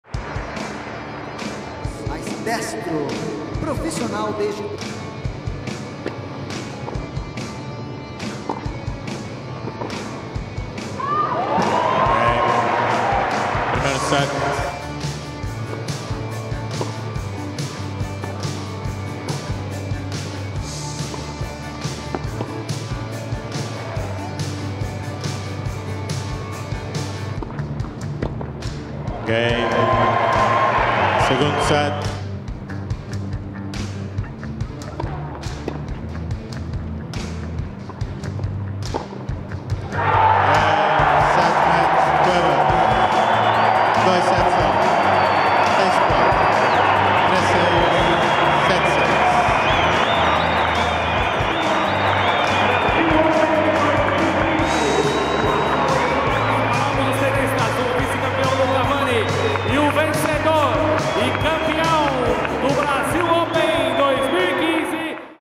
Sao Paulo 2015 Final Highlights Cuevas Vanni
Watch highlights of the Brasil Open 2015 final, featuring Pablo Cuevas and Luca Vanni.